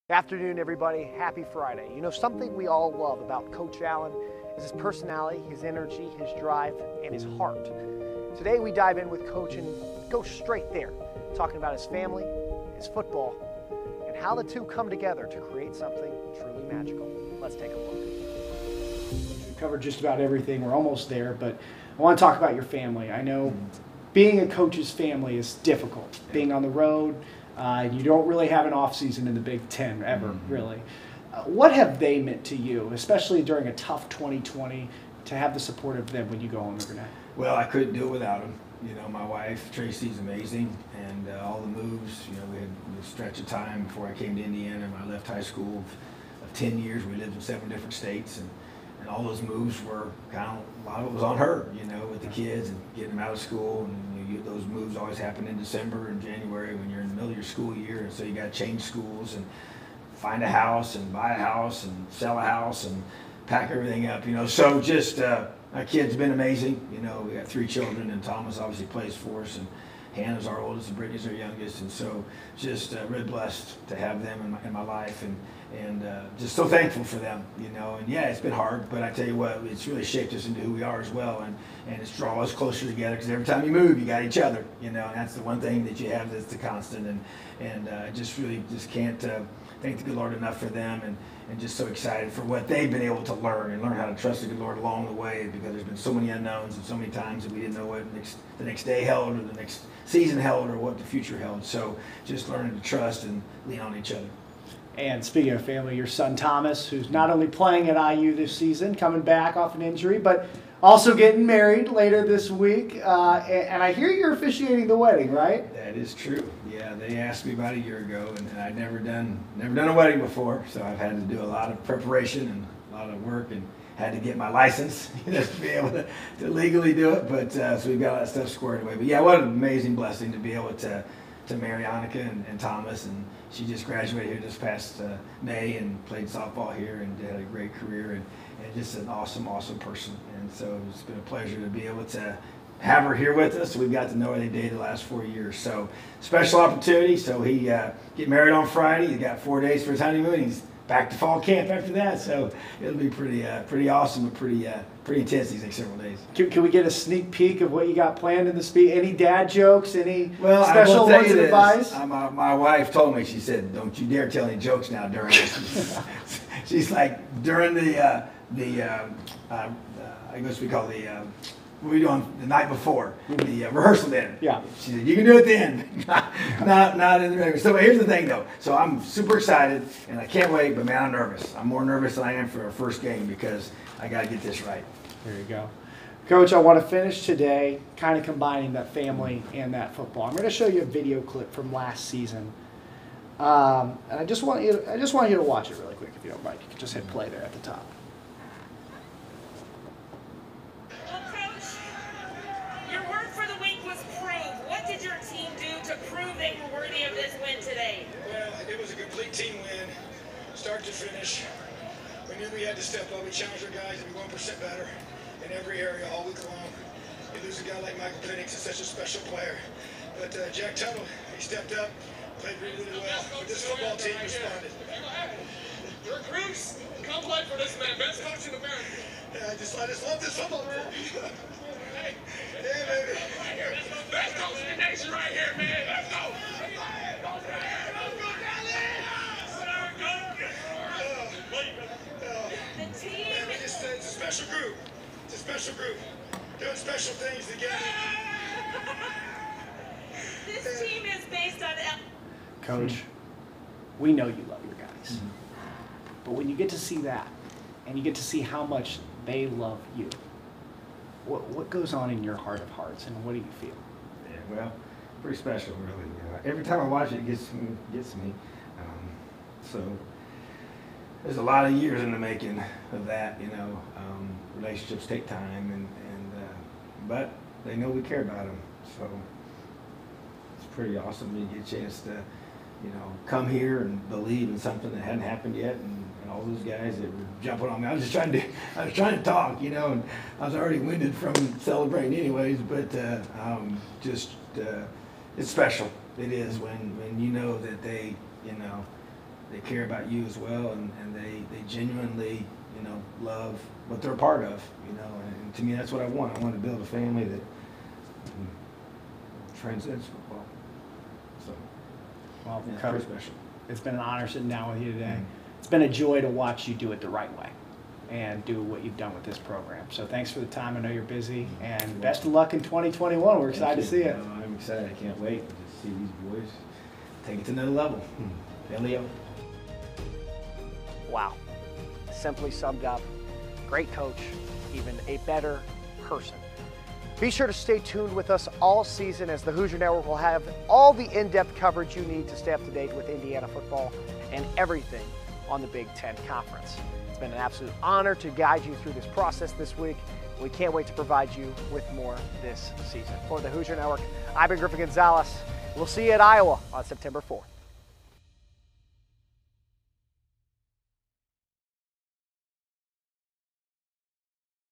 The Sitdown: Tom Allen (Part 3 of 3)
Join Griffin Gonzalez as he sits down with Indiana Head Football Coach Tom Allen in an exclusive 3-part interview series centered around the new wave surrounding Indiana Football.